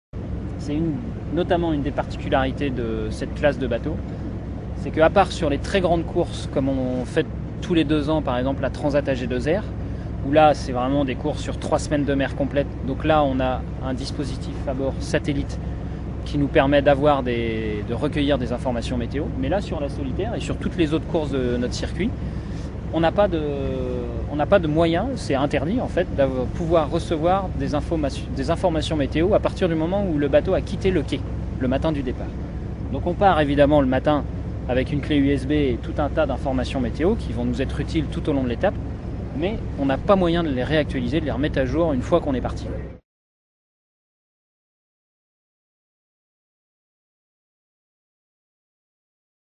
Nicolas Lunven (Generali) : "sur la Solitaire, il est interdit de recevoir des informations météo"
Toute la semaine, les skippers de la Solitaire du Figaro effectuent des "runs" d'exhibition sur la Garonne à Bordeaux. Nous voici sur le monotype de Nicolas Lunven (Generali), l'un des favoris de la course